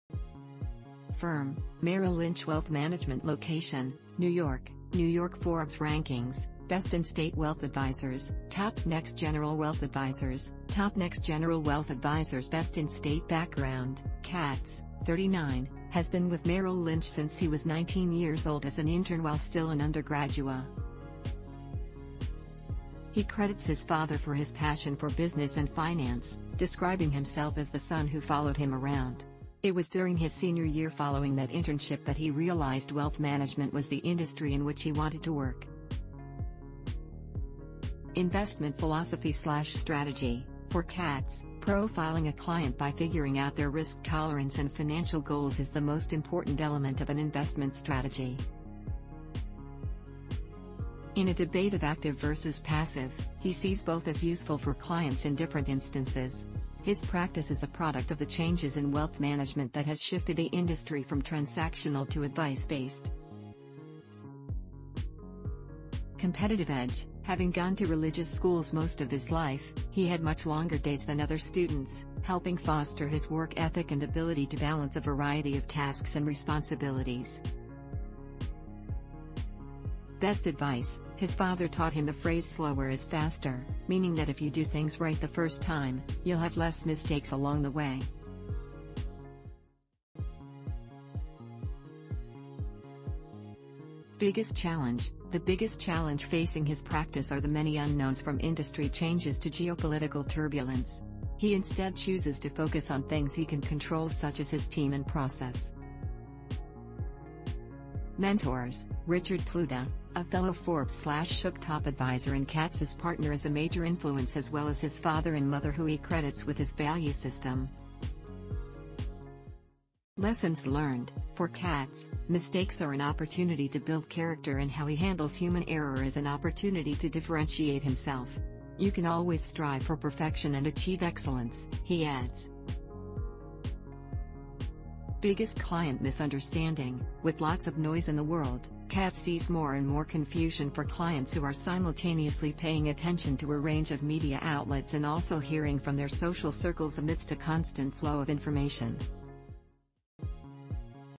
Merrill Lynch Advisor Uses Discipline Learned In Religious School To
Firm: Merrill Lynch Wealth ManagementLocation: New York, New YorkForbes Rankings: Best-In-State Wealth Advisors, Top Next-Gen Wealth Advisors, Top Next-Gen Wealth Advisors Best-In-StateBackground: Katz, 39, has been with Merrill Lynch since he was 19 years old as an intern while still an undergraduate at the Yeshiva University Sy Syms School of Business. It was during his senior year following that internship that he realized wealth management was the industry in which he wanted to work. Investment Philosophy/Strategy: For Katz, profiling a client by figuring out their risk tolerance and financial goals is the most important element of an investment strategy. In a debate of active versus passive, he sees both as useful for clients in different instances. His practice is a product of the changes in wealth management that has shifted the industry from transactional to advice-based. Competitive Edge: Having gone to religious schools most of his life, he had much longer days than other students, helping foster his work ethic and ability to balance a variety of tasks and responsibilities. Best Advice: His father taught him the phrase “slower is faster,” meaning that if you do things right the first time, you’ll have less mistakes along the way. Biggest Challenge: The biggest challenge facing his practice are the many unknowns from industry changes to geopolitical turbulence. He instead chooses to focus on things he can control such as his team and process. Mentors: Richard Pluta, a fellow Forbes/SHOOK Top Advisor and Katz’s partner is a major influence as well as his father and mother who he credits with his value system. Lessons Learned: For Katz, mistakes are an opportunity to build character and how he handles human error is an opportunity to differentiate himself. “You can always strive for perfection and achieve excellence,” he adds. Biggest Client Misunderstanding: With lots of noise in the world, Katz sees more and more confusion for clients who are simultaneously paying attention to a range of media outlets and also hearing from their social circles amidst a constant flow of information. Investment Outlook: Katz is “not here to predict the markets,” instead looking to educate clients on the front end sufficiently so that they are prepared for market environments that are both bearish and bullish.“You have to be transparent with clients and explain what to expect out of each asset class and the appropriate time horizon,” he adds. Katz looks to keep clients focused on objectives and not to predict or look to time markets.“I have the luxury of speaking with some premier asset managers and garnering some of the best perspectives from some of the best intellectual capital in the world.